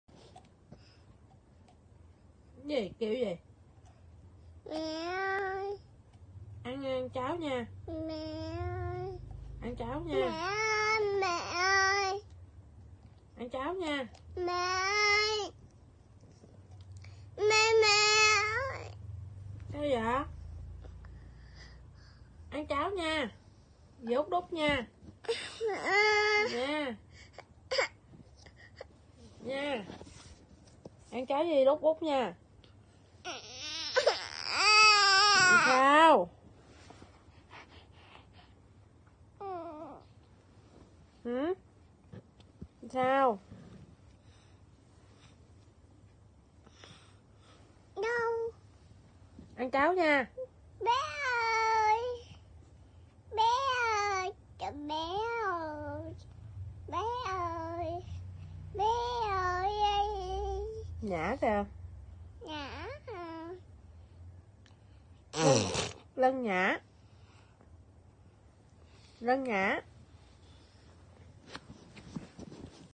Mẹ vắng nhà em nhớ mẹ
Video út quay ngày 13/12/2019 mẹ đi gởi hàng ở Giao hàng Tiết Kiệm Phan Văn Trị bị ăn cắp mất chiếc Wave Anpha đời 2017